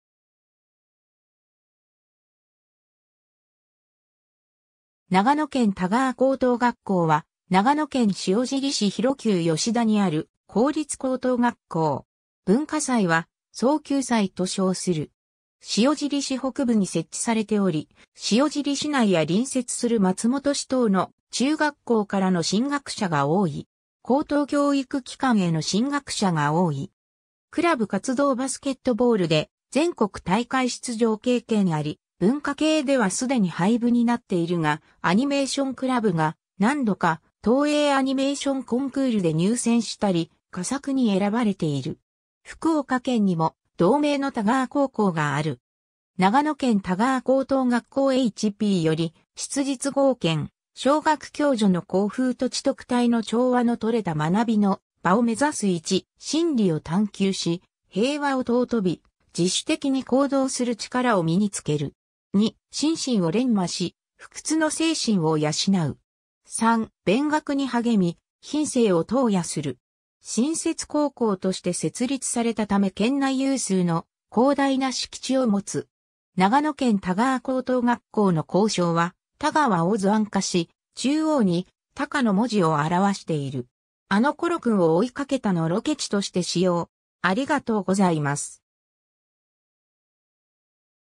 長野県田川高等学校
長野県田川高等学校（ながのけん たがわこうとうがっこう）は、長野県塩尻市広丘吉田にある公立高等学校。
 文化祭は「蒼穹祭」と称する。
 塩尻市北部に設置されており、塩尻市内や隣接する松本市等の中学校からの進学者が多い。
高等教育機関への進学者が多い。
 クラブ活動バスケットボールで全国大会出場経験あり、文化系ではすでに廃部になっているが、アニメーションクラブが、何度か東映アニメーションコンクールで入選したり、佳作に選ばれている。
 福岡県にも同名の田川高校がある。
長野県田川高等学校HPより（ 『質実剛健、尚学共助の校風と知徳体の調和のとれた学びの場を目指す』 １．真理を探究し、平和を尊び、自主的に行動する力を身につける。
 ２．身心を錬磨し、不屈の精神を養う。
 ３．勉学に励み、品性を陶冶する。
 新設高校として設立されたため県内有数の広大な敷地を持つ。
 長野県田川高等学校の校章は、「田川」を図案化し、中央に高の文字を表している。
 ※あの頃君を追いかけたのロケ地として使用